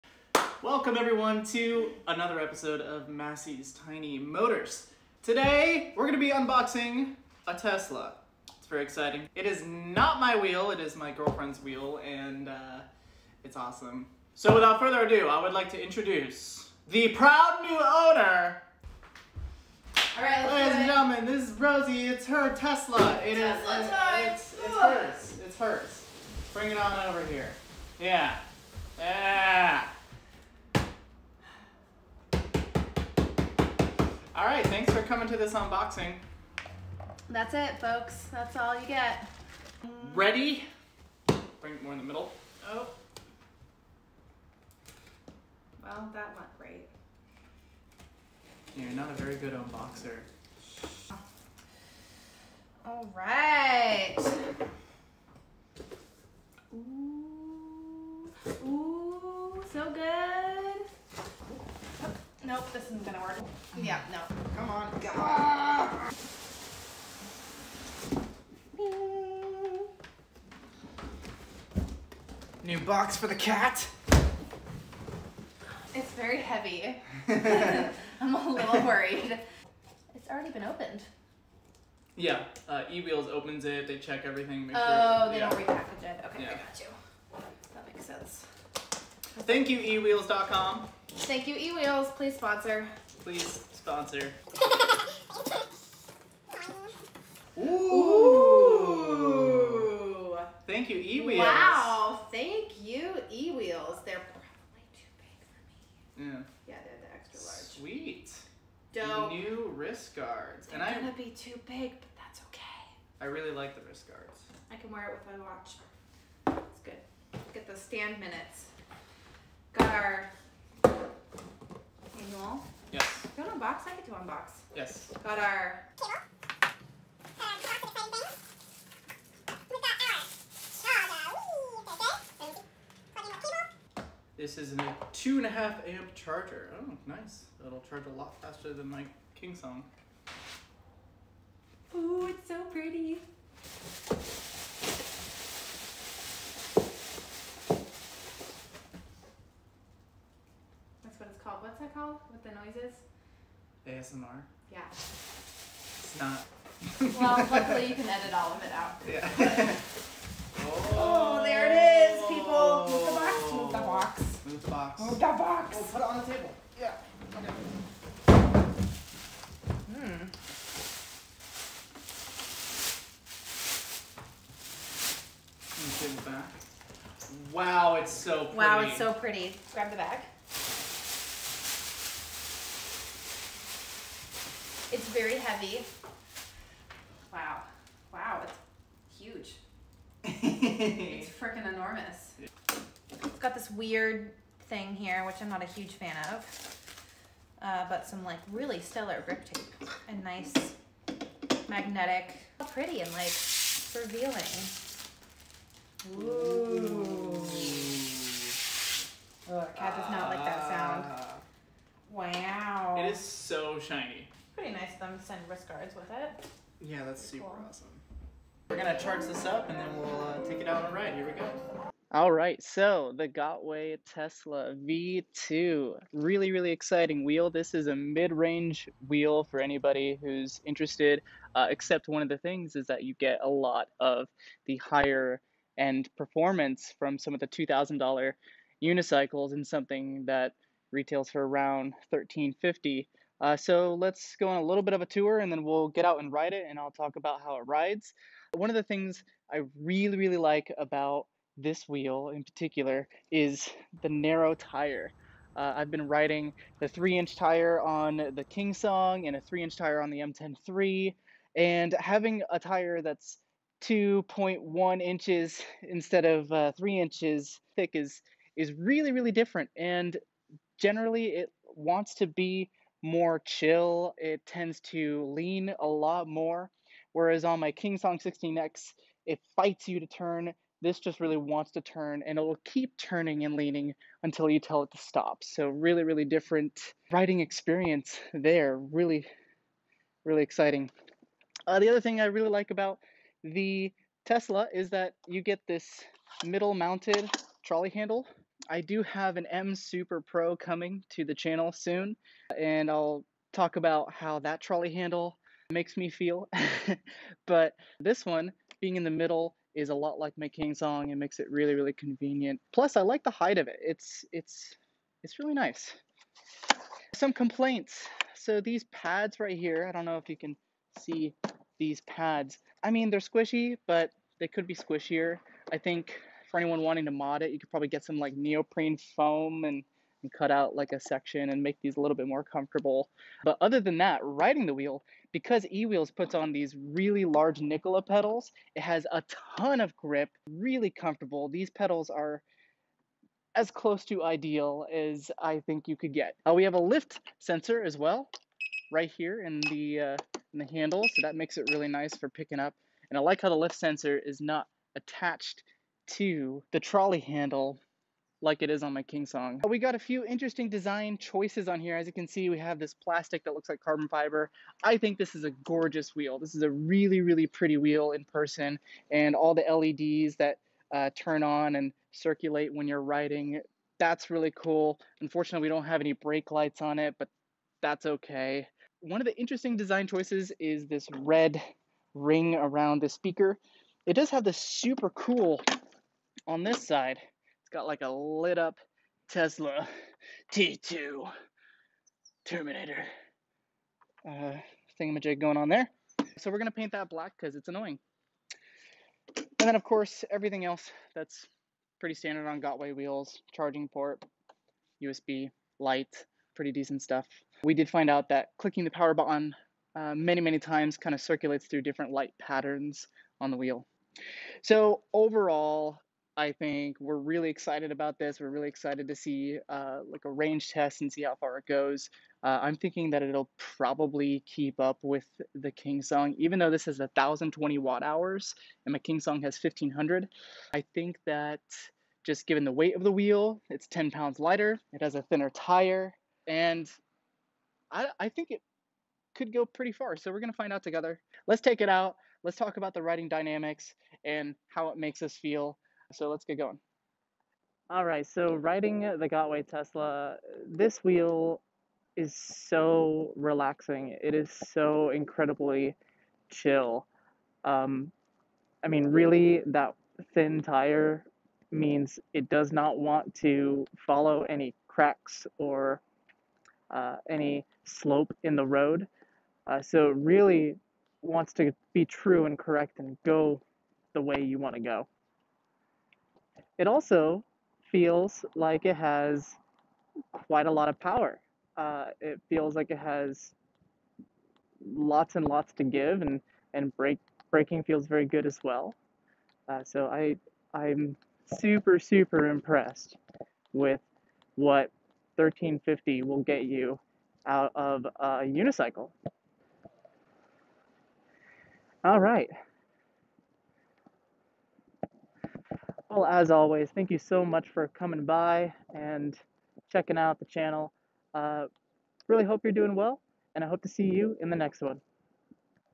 Tesla unboxing and initial impressions
In this vide we unbox the Gotway Tesla V2 and share some thoughts!